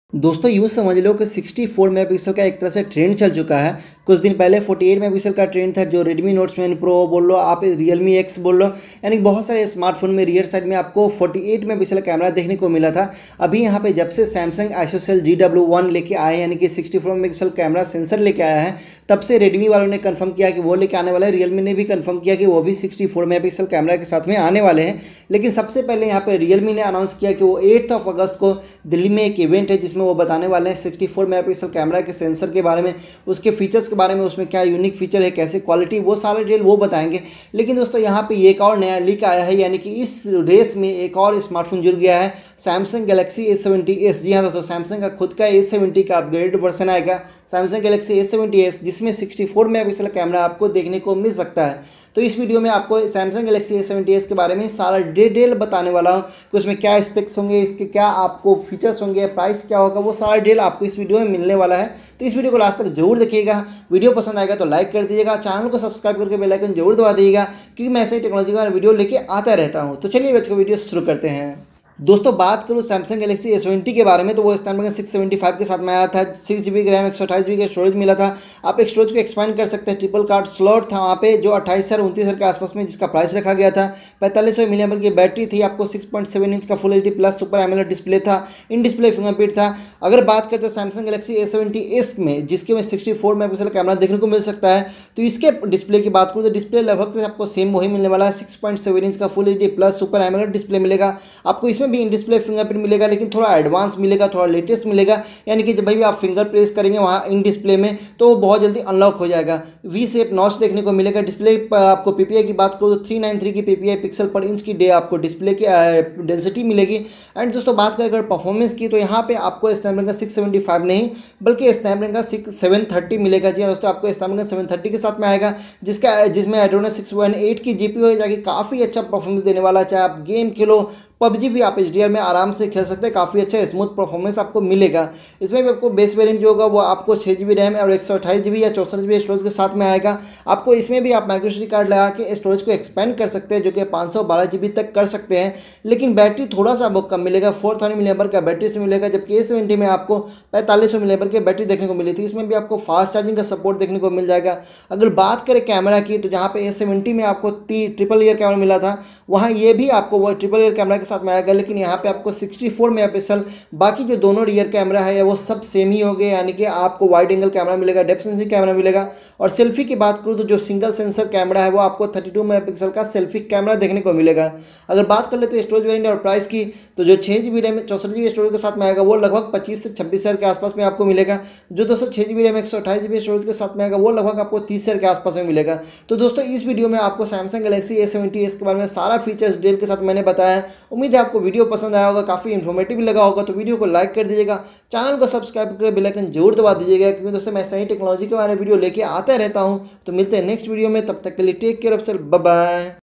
Samsung Galaxy A70s Specifications, Price And Launch Date In India
Hello Dosto Aapsab Kaise Ho?
 In This Video I Have Explained That According Rumors In Samsung Galaxy A70s Will Have 64MP Camera Censor. Samsung Galaxy a70s Launch Date In India. Samsung Galaxy A70s  Price In India. Samsung Galaxy a70s full Details.